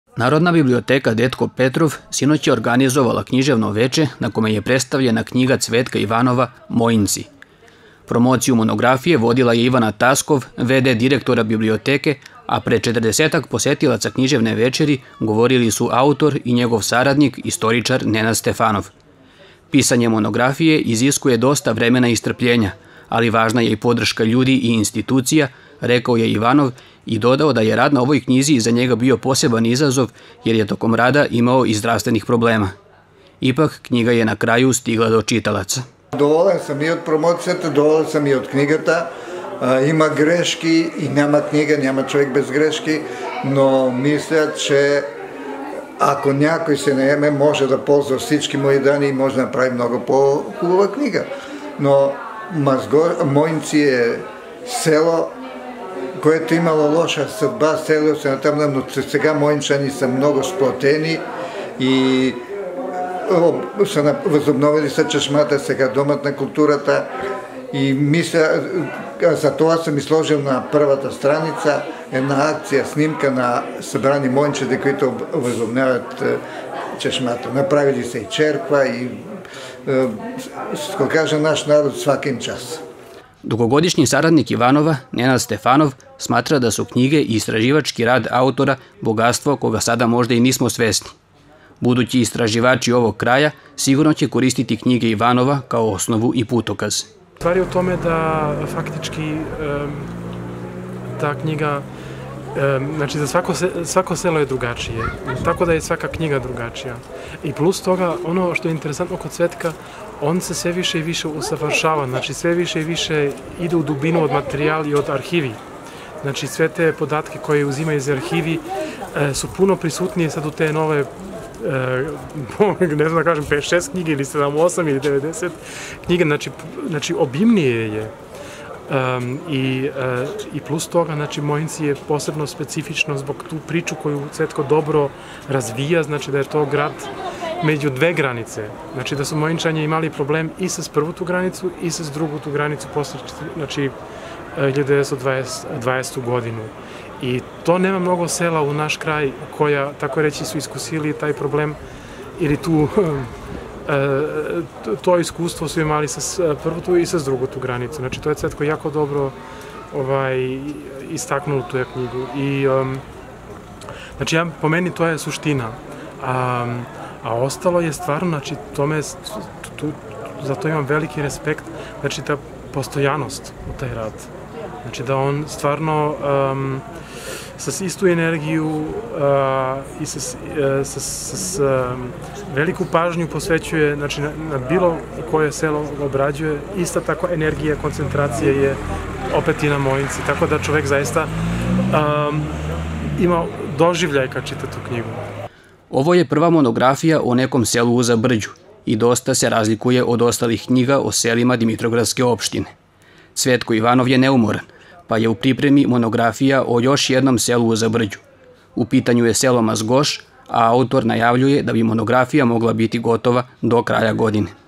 ODRŽANA PROMOCIJA KNJIGE “MOJINCI” AUTORA CVETKA IVANOVA 22.08.2024. PRILOG
ODRŽANA PROMOCIJA KNJIGE “MOJINCI” AUTORA CVETKA IVANOVA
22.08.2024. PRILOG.